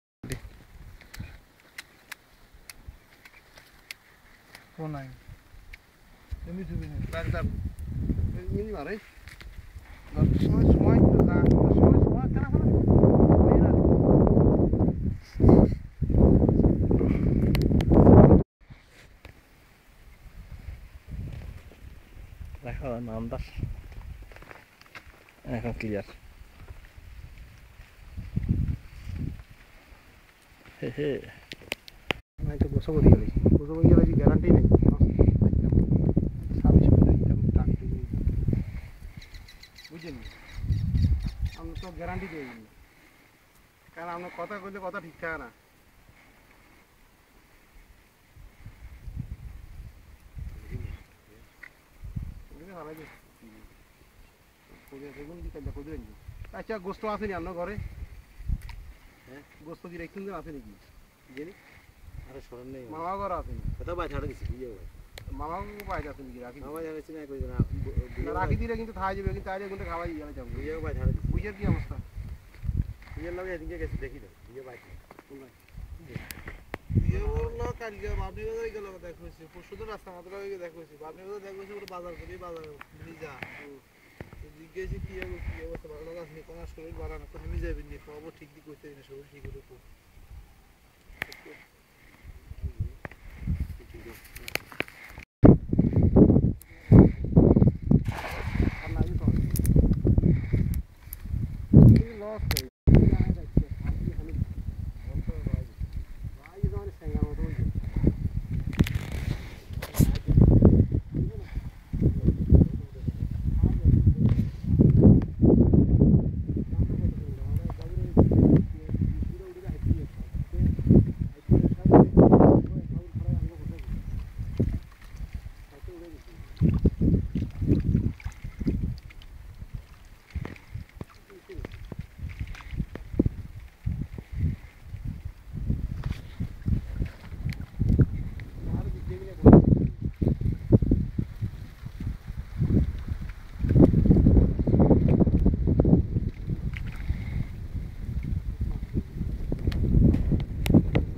king prawn (anwar from ferryhill)
hi this is anwar from ferryhill uk
this is our big pond and there are lots of live king prawns, we sell them in the market